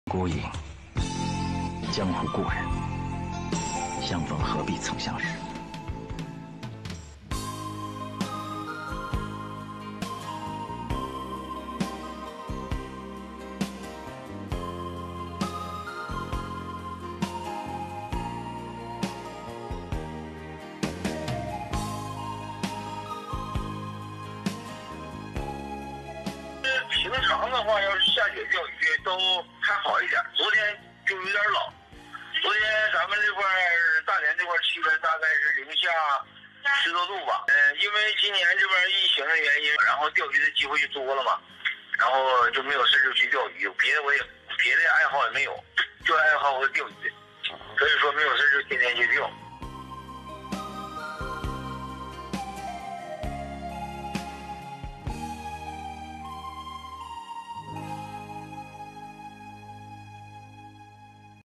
Man in Dalian goes viral for fishing in heavy snow calmly
A man in Dalian, Liaoning went viral for fishing alone in heavy snow calmly on Jan. 6. He says that fishing was his one and only passion, and he goes fishing every day regardless of the weather.